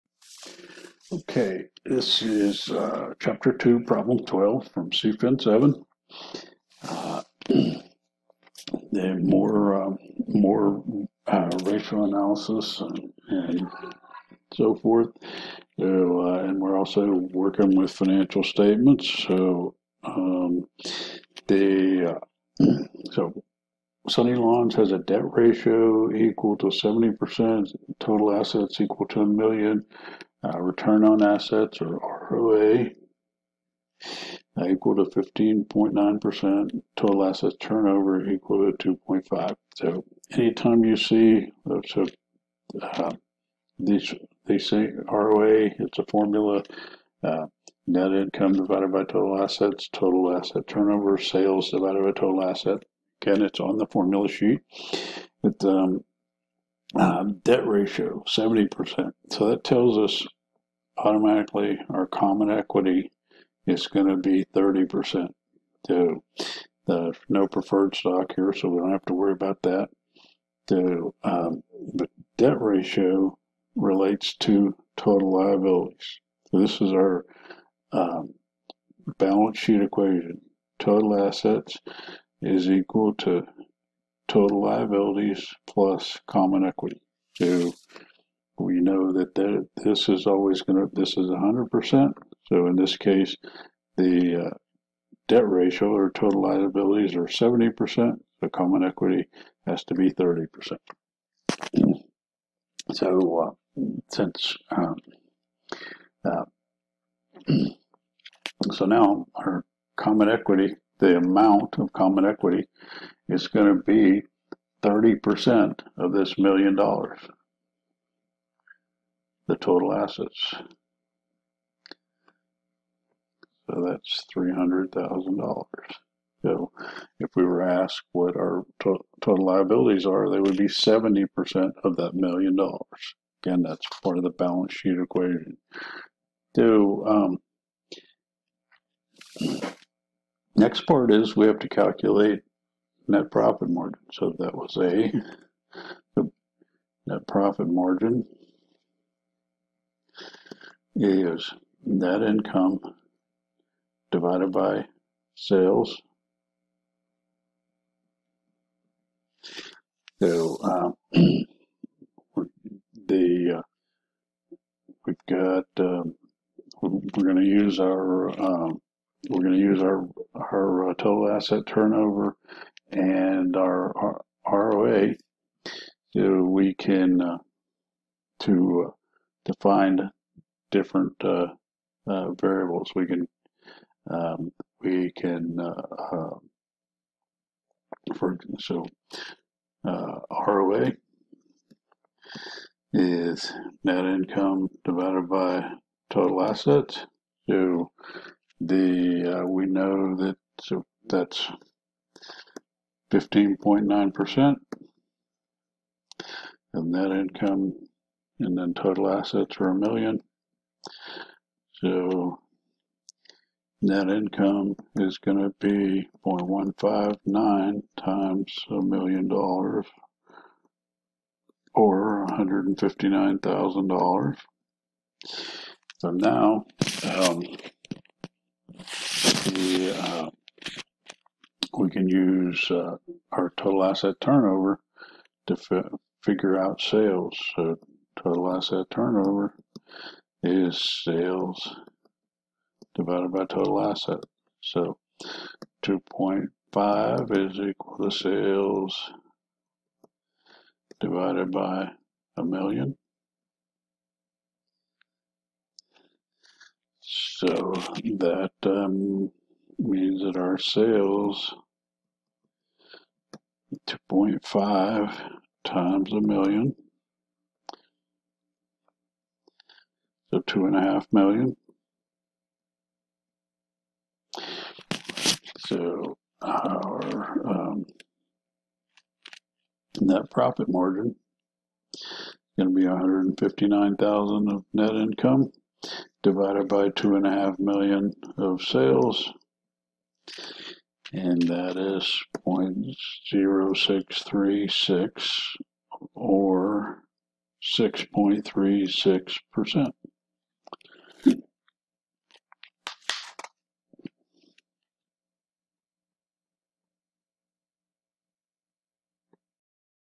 Problem 2 12 CFIN7
Part (a) of this problem uses Total Assets and the Debt Ratio to calculate Common Equity.  Part (b) uses ROA and Total Assets to calculate Net Income, uses TAT and Total Assets to calculate Sales and uses Net Income and Sales to calculate NPM.